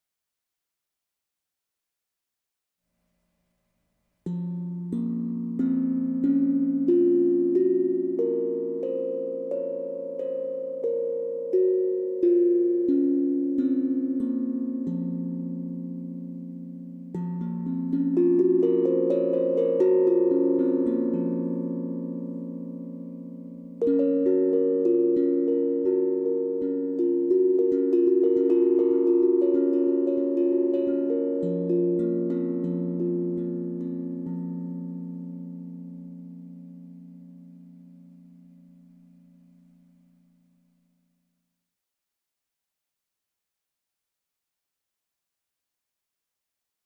OVNI Drum - Escala Akebono en Fa - Steel Tongue Nektar not Hang
OVNI Drum - Escala: Akebono en Fa (origen: Japon) - Pintura Roja Cereza - Instrumento Metálico de Percusión Melódica - Clásico de 9 notas con tono grave central. Steel Tongue Drum.